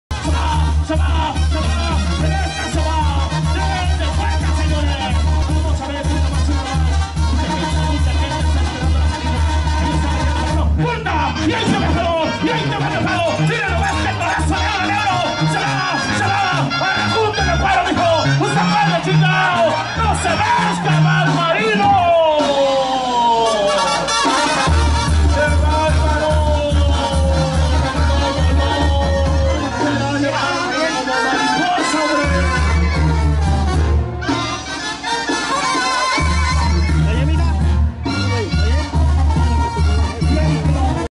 EL GALLITO VS EL INDIO ENAMORADO
Toro: El Gallito
Jinete: El Indio Enamorado
Rancho San Juditas
Torneo de toros Santos Reyes Nopala 2021